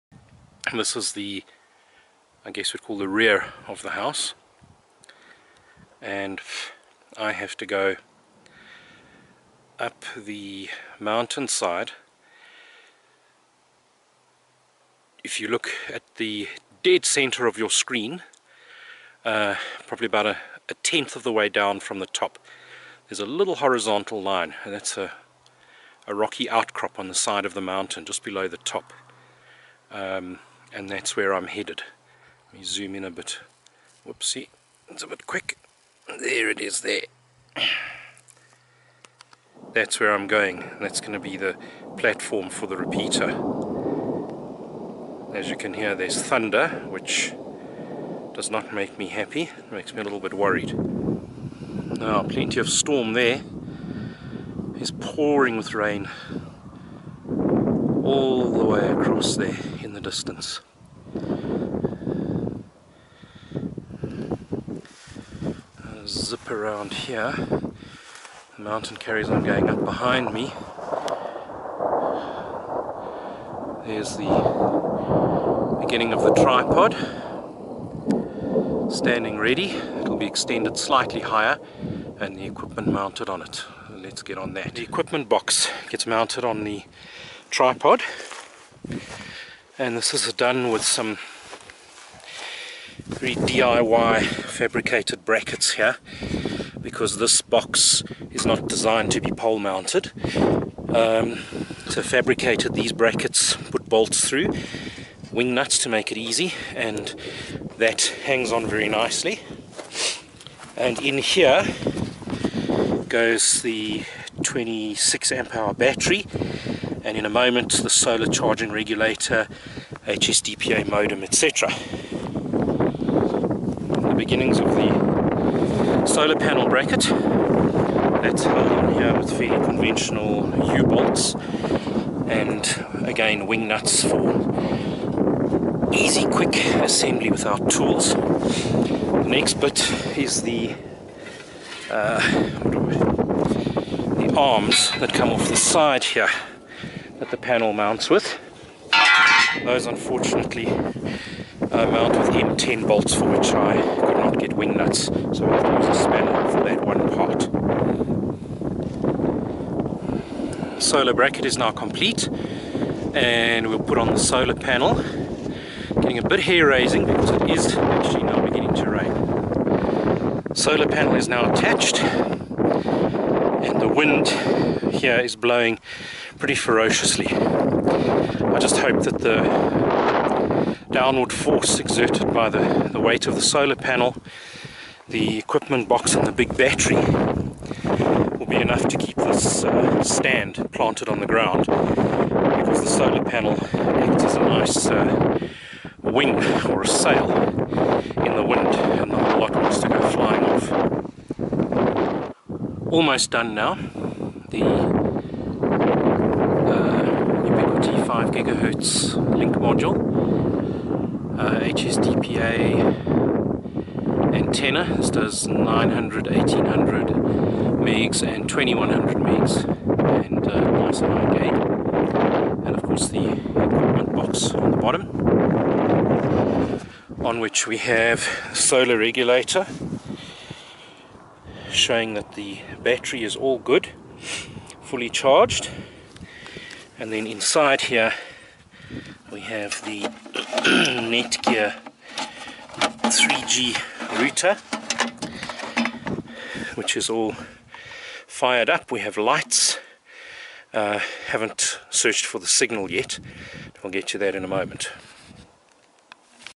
3G repeater assembly
Putting together the 3G repeater on a mountain